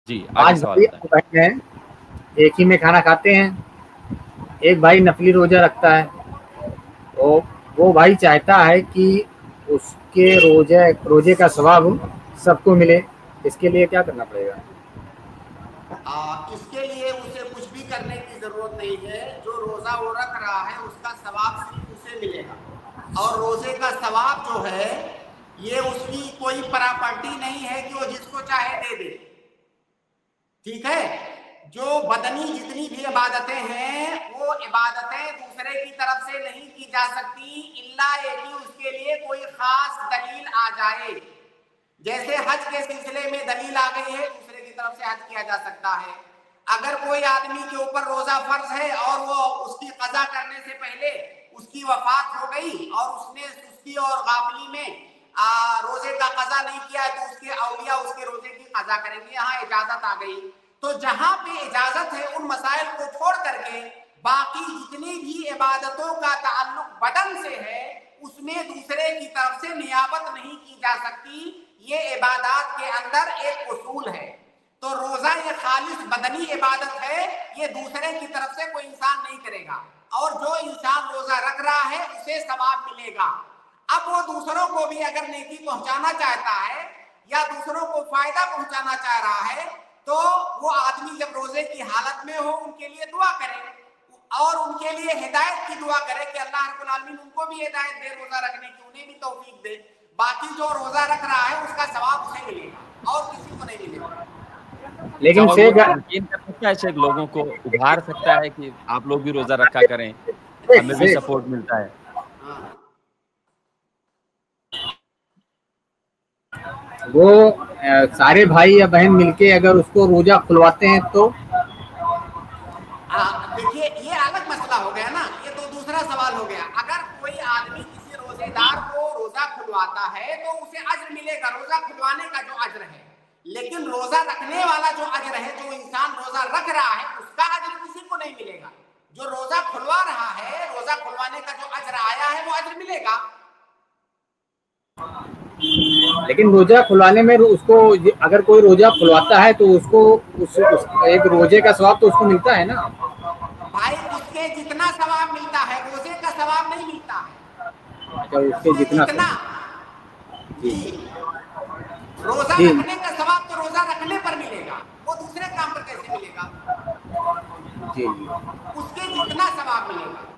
Nafli Roza Rakhne Wale Ka Sawab Sabko Mile, Iske Liye Kya Karna Hoga? by Dr. Mohammad Naseem
Nafli Roza Rakhne Wale Ka Sawab Sabko Mile, Iske Liye Kya Karna Hoga? by Dr. Mohammad Naseem Madani iPlus TV

UPI : donateilm@icici